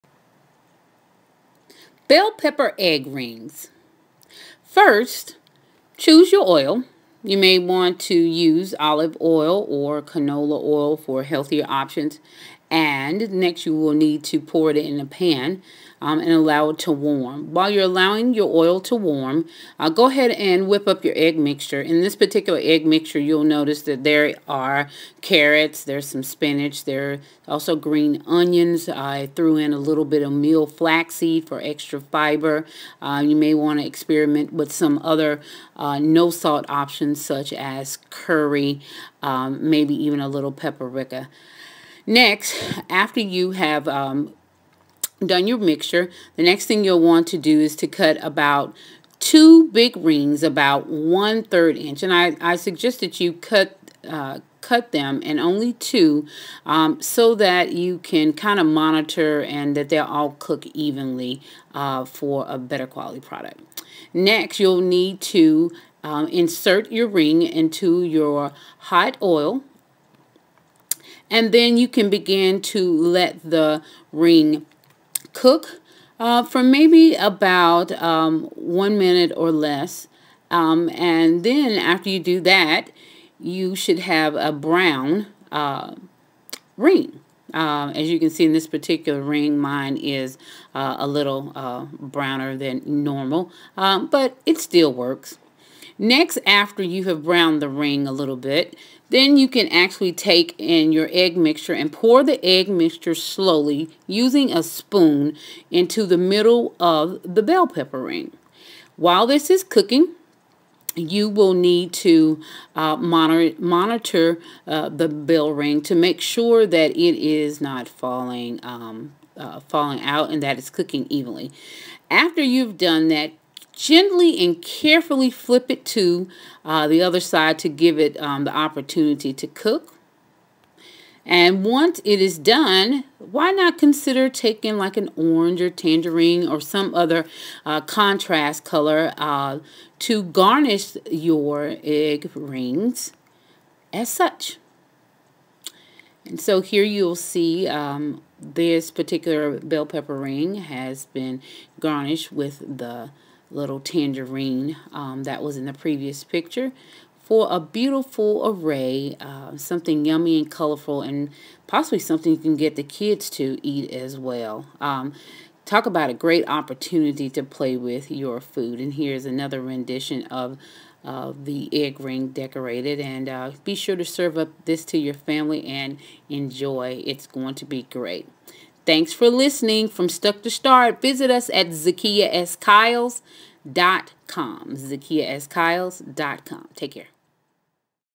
Bell-pepper rings
Bellpepper Egg Rings are a great twist on breakfast. They are quick and easy to prepare. Whip up some of these for someone you love. Smooches!